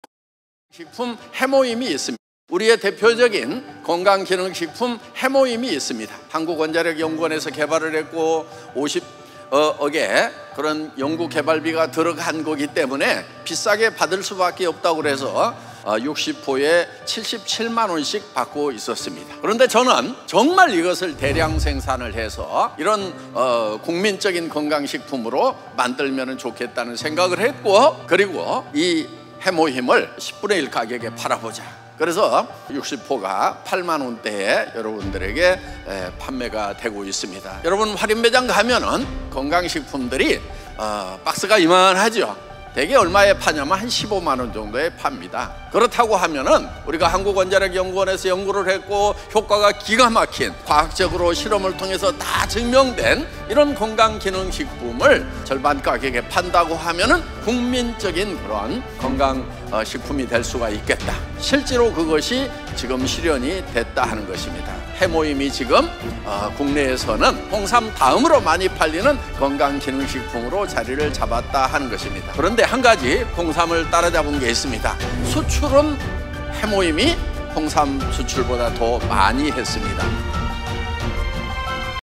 애터미 헤모힘 건강기능식품 수출액 1위!
60포에 77만원 하던 헤모힘을 대량생산하여 국민건강식품으로 만든 애터미!
건강기능식품 수출 품목 1위까지 달성!

※특허승인!!'
면역력 증진
혈액순환 개선
간기능 개선
피로회복 개선
운동수행능력 증진

누적매출 2조 돌파!
단일품목으로 21개국 국제특허 취득!